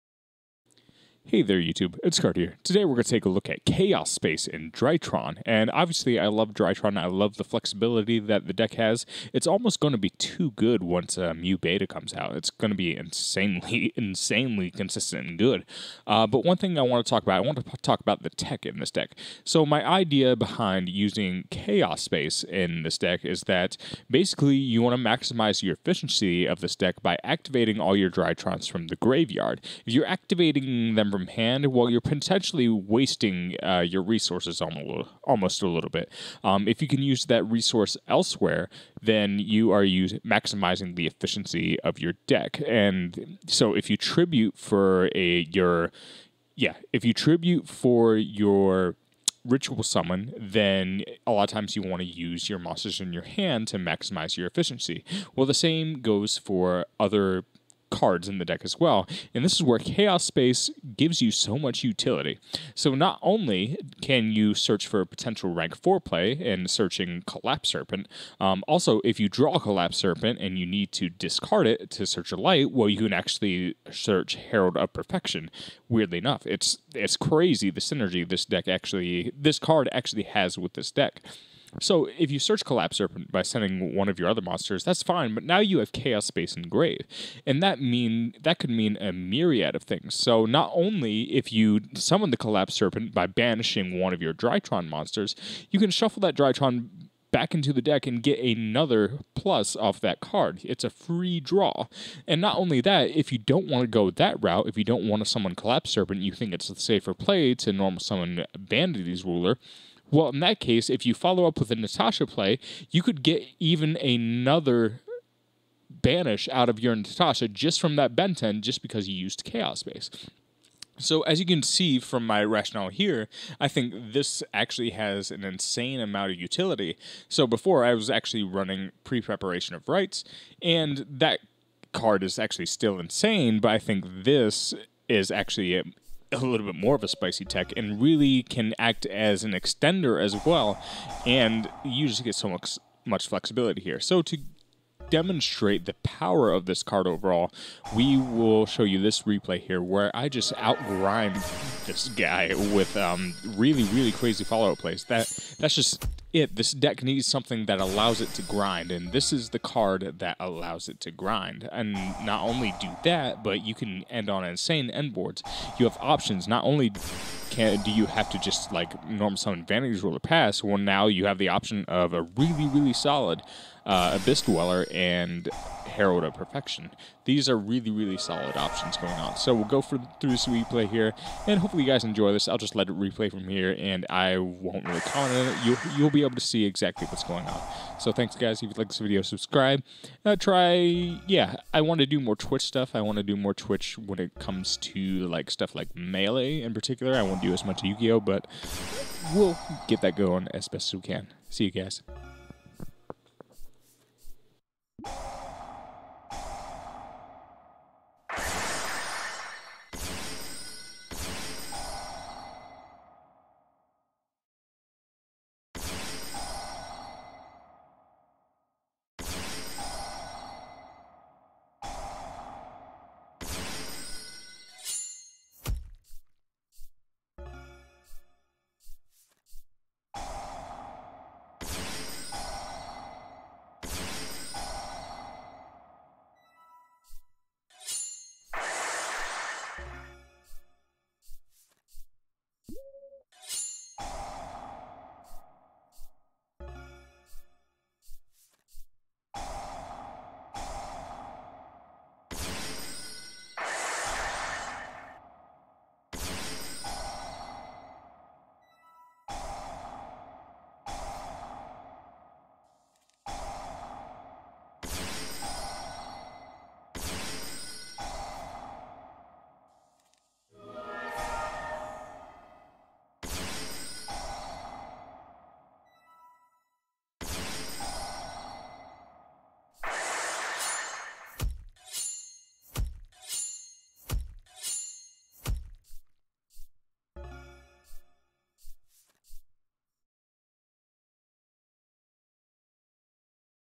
Chaos Space in DRYTRON is Filthy Good
The card helps to round out some of the deck's weaknesses.